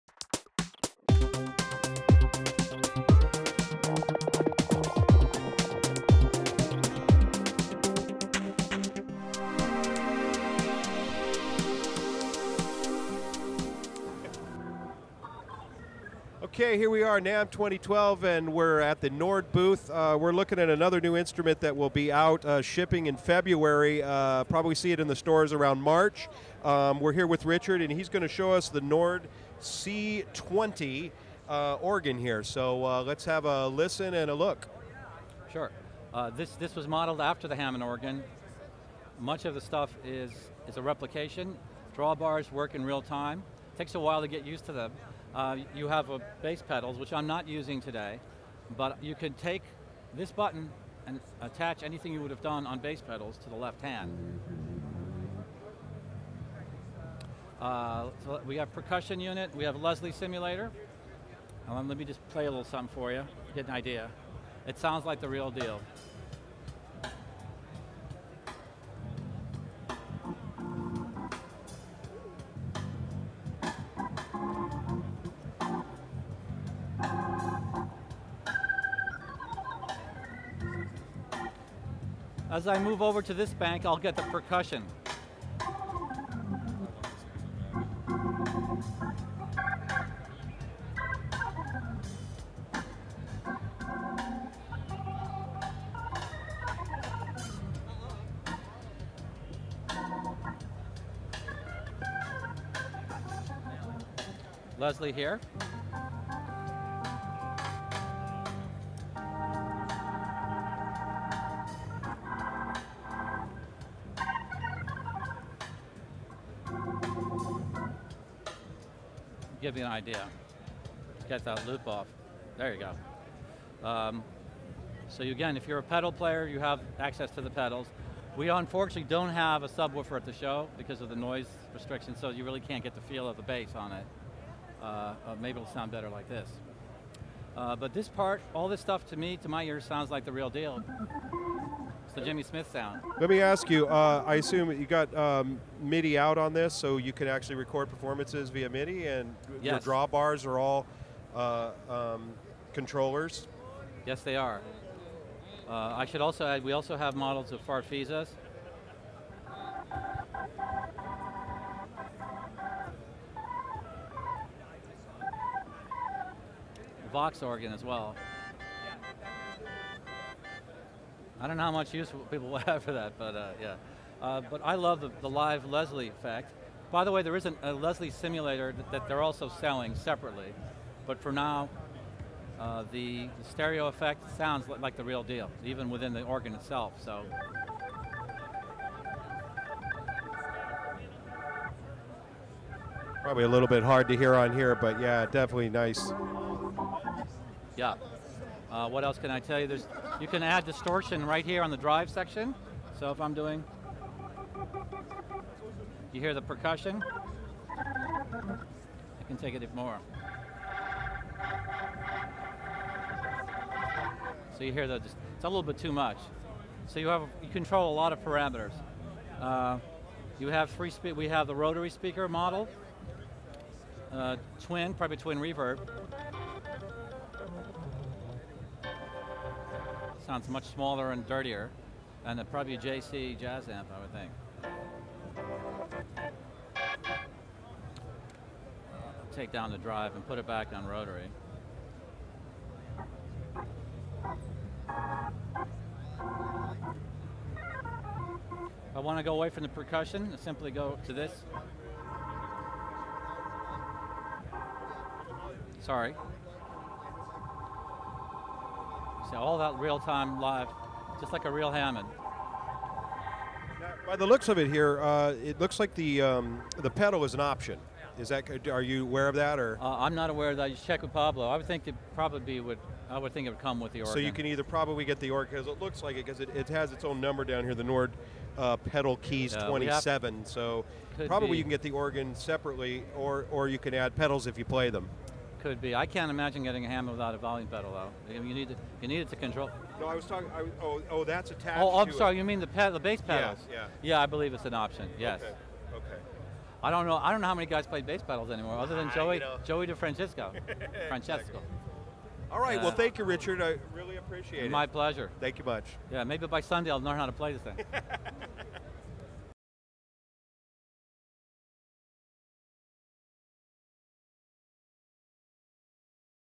New Nord C2D Organ Musicianews Namm 2012
Richard shows us the new Nord C2D Organ which will be released Feb/Mar 2012